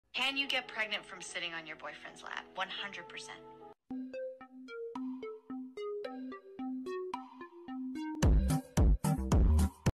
“Can you get pregnant from sitting on your boyfriend’s lap?” Meme | Helliam | Gacha Club
Warning;
My AU
Henry x William
mpreg - (m)ale (preg)nancy